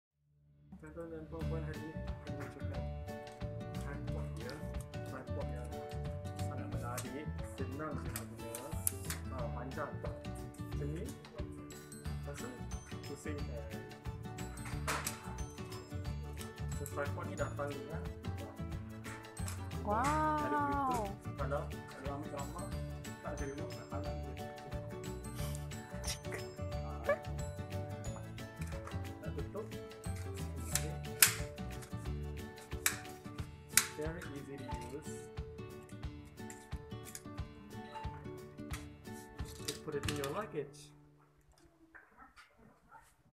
40 seconds to Lock and Unlock Tripod Challenge!! 🥳🥳😍😍😍Cuba kalau dapat!
A 40 seconds video on how to lock and unlock your tripod. RM30 only with a device that can control from afar 😍😍 Love it! Thanks to my collegue for being giving us a tutorial!